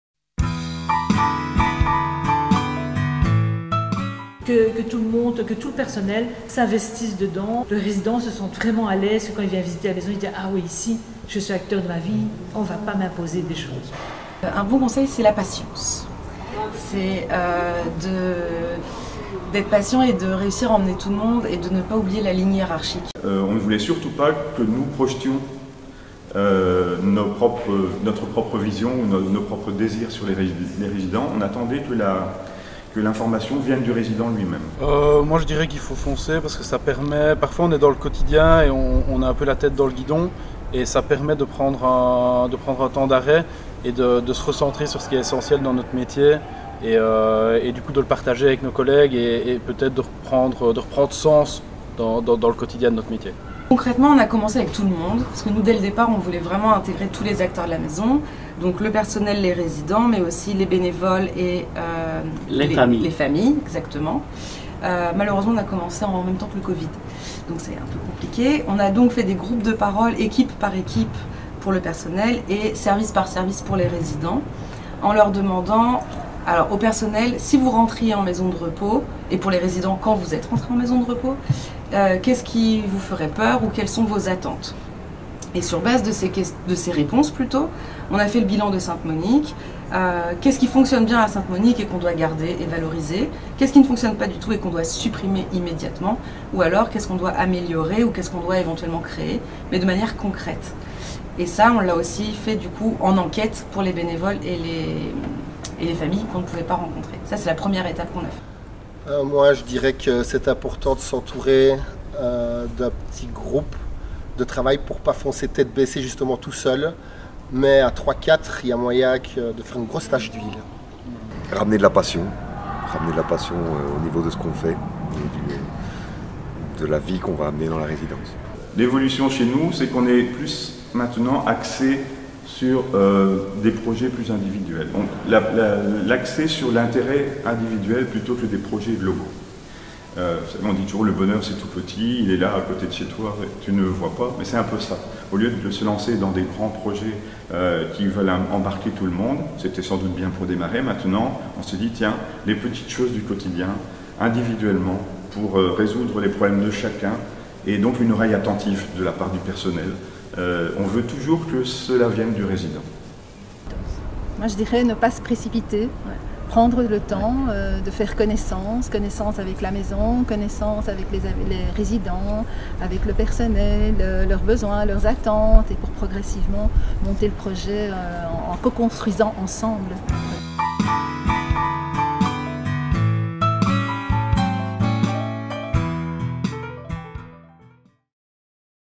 Tubbe – Quelques conseils pour démarrer
Quelques conseils pour se lancer dans l’aventure et pour implémenter la philosophie Tubbe au sein de votre maison de repos (et de soins).